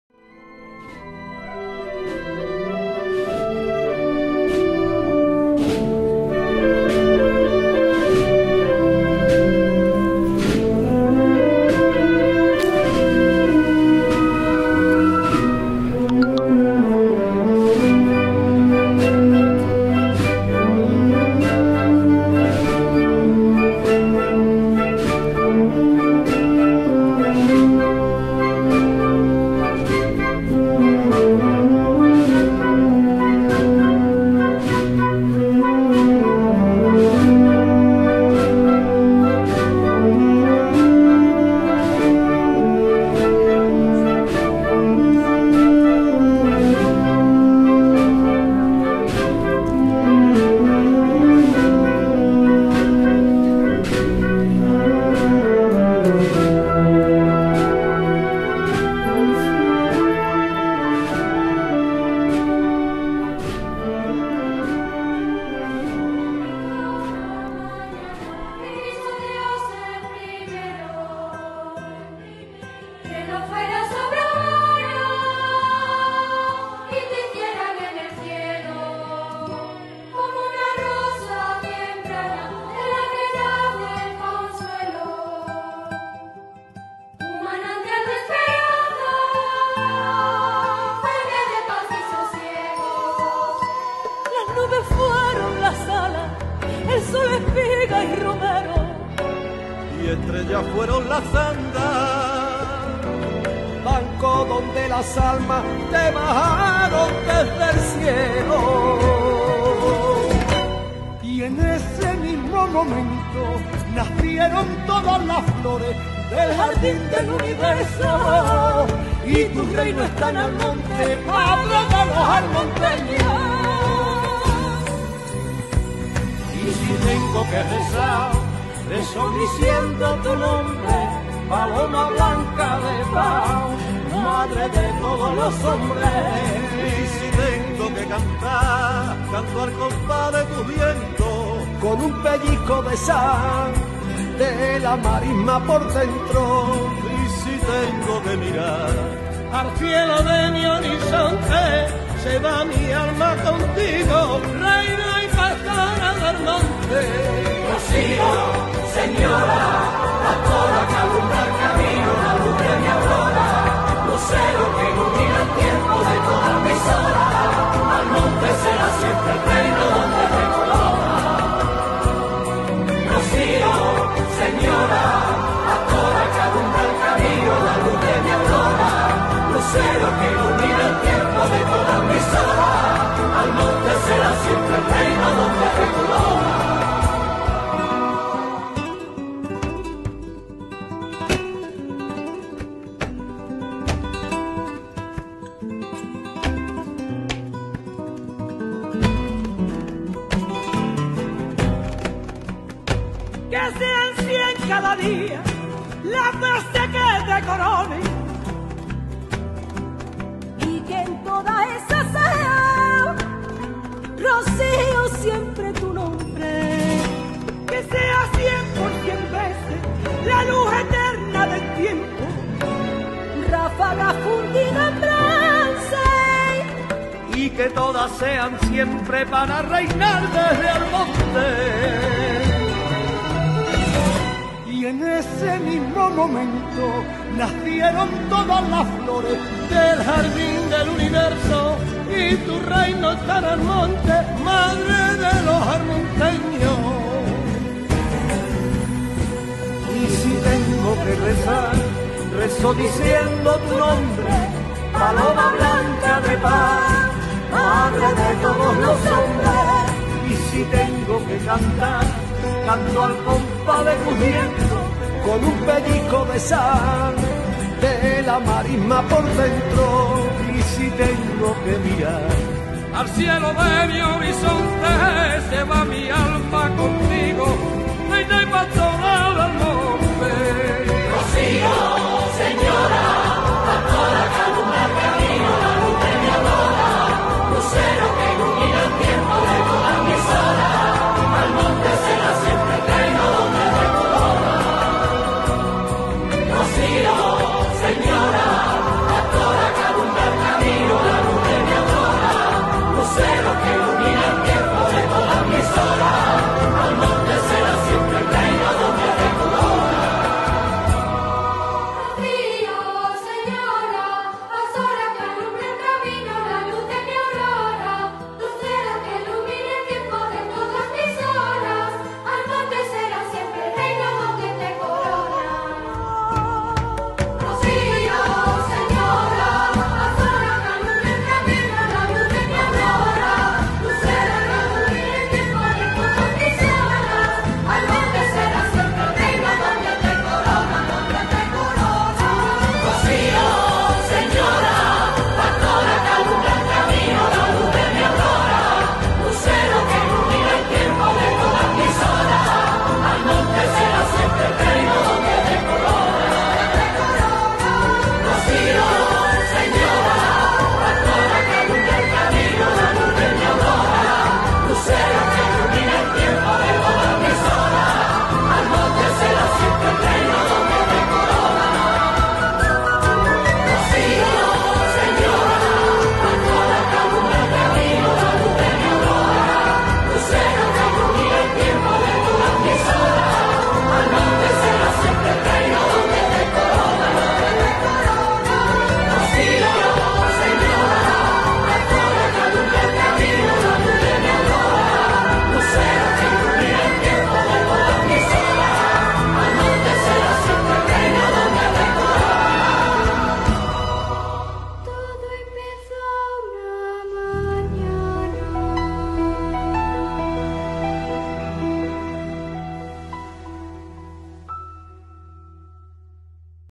4K - NTRA SEÑORA DEL ROCÍO EN ALMONTE 2020 HUELVA
El Canal Cofrade Esperanza y Pasión visita a Ntra Señora del Rocío en la Iglesia de Nuestra Señora de la Asunción de Almonte. Lugar en el que se encuentra desde el 20 de Agosto de 2019 y no ha podido regresar hasta la aldea del Rocío por causa de la Pandemia.